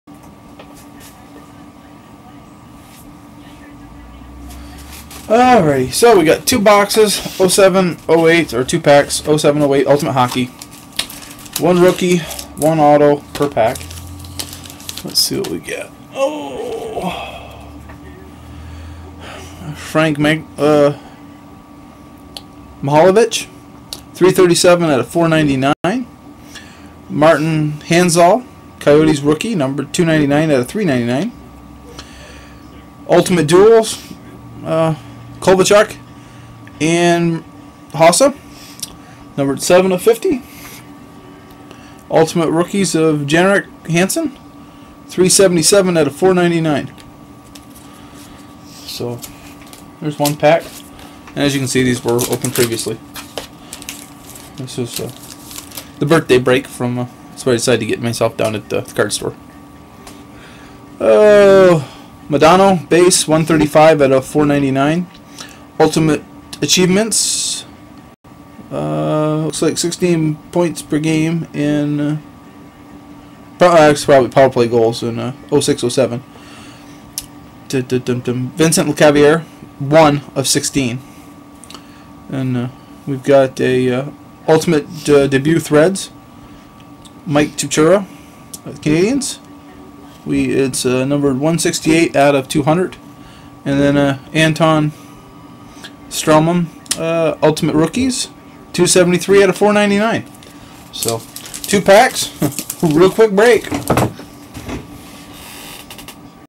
2 Packs of 0708 Ultimate Hockey
Pack # 1:
Mike Modano Base /499
Ultimate Achievements Vincent Lecavalier Auto 1/16
Ultimate Debut Threads Kyle Chipchura /200
Ultimate Rookies Anton Stralman /499

Pack # 2:
Frank Mahovlich Base /499
Martin Hanzal Ultimate Rookie Autograph /399
Ultimate Duos Dual GU JSY Kovalchuk/Hossa (Trashers) 7/50
Jannik Hansen Ultimate Rookies /499